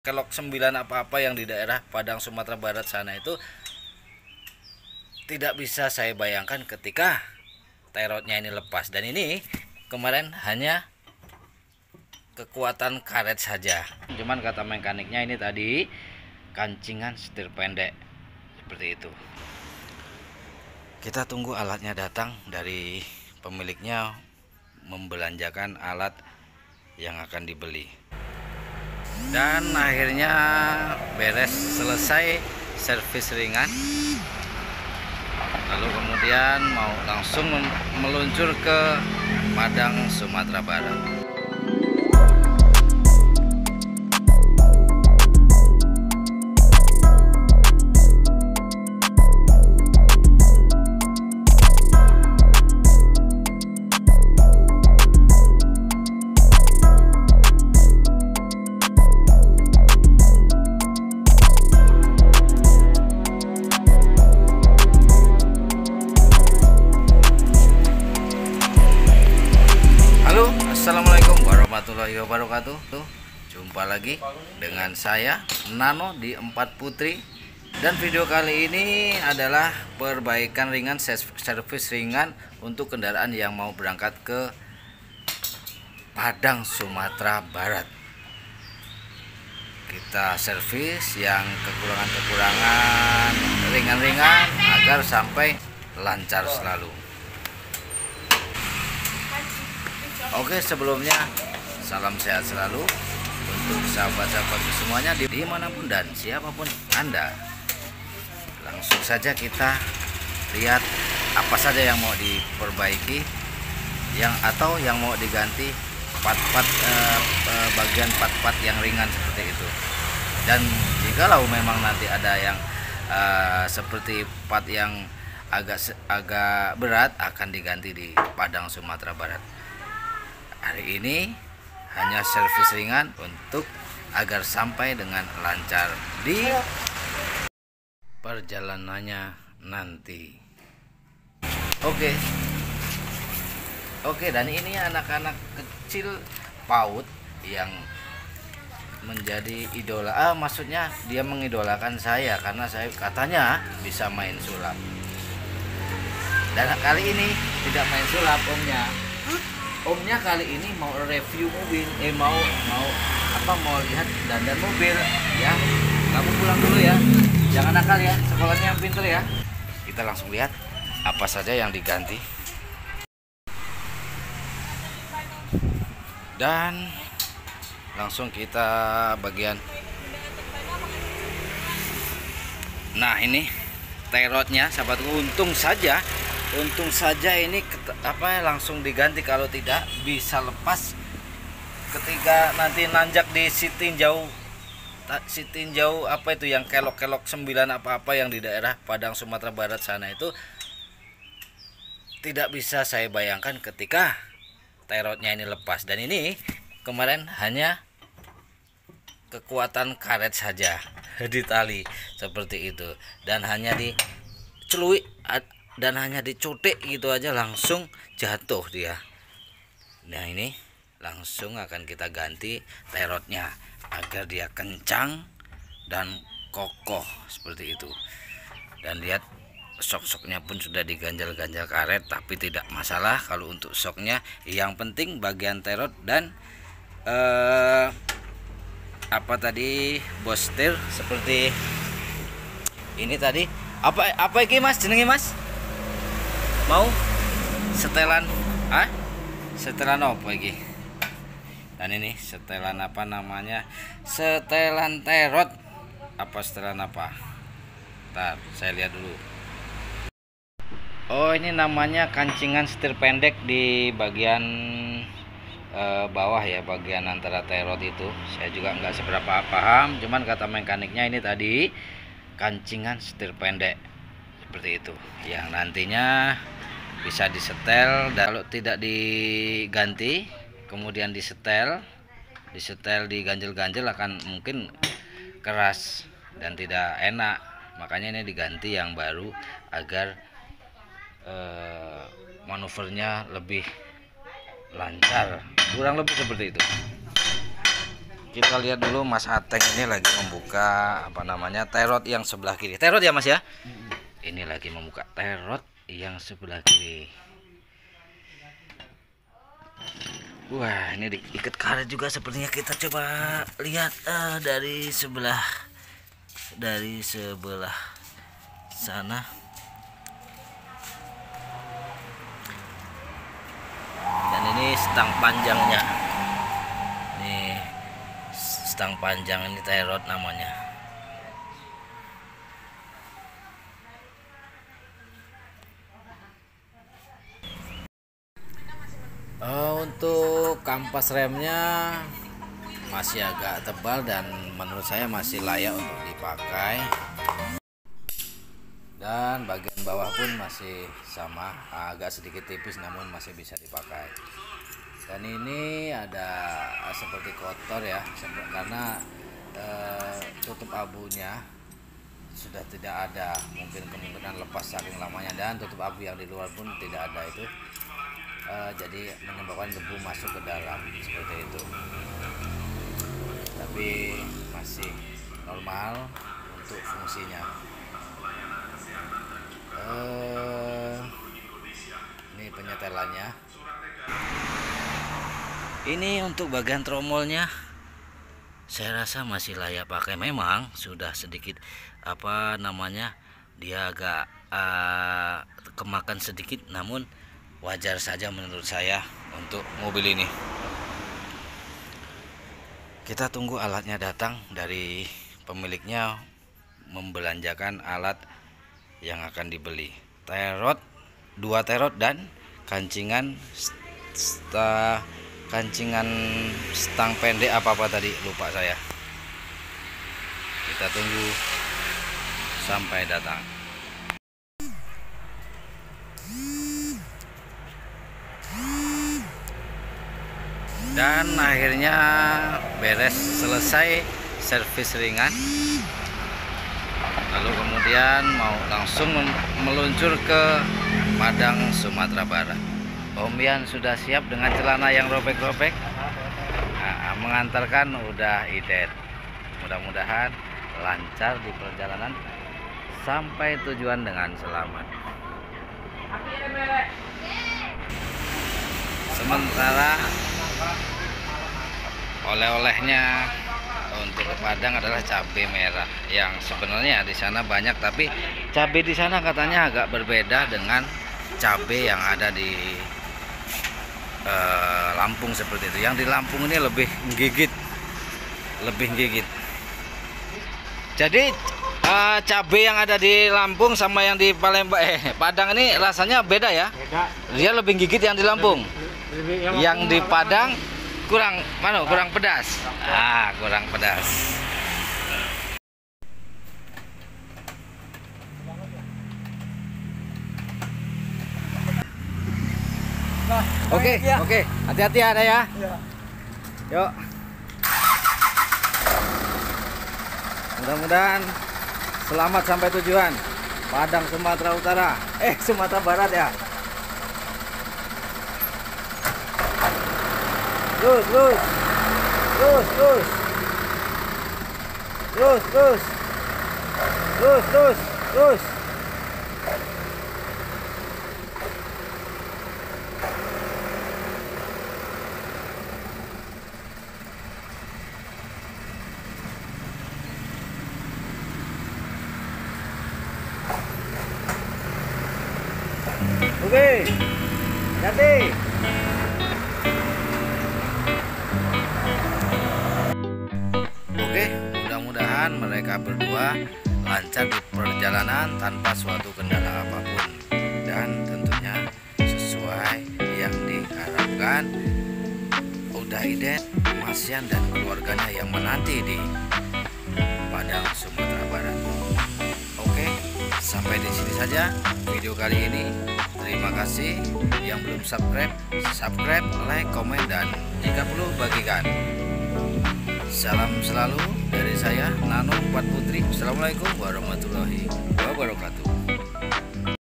Mitsubishi ps100 1990 reot‼️otw Padang service dulu
Mitsubishi ps100 reot otw Padang Sumatra barat semoga lancar dan menghibur...

info pesan mobil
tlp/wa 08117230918

tag;
Mitsubishi ps100 otw Padang,service dulu sebelum jalan jauh,truk tua handal,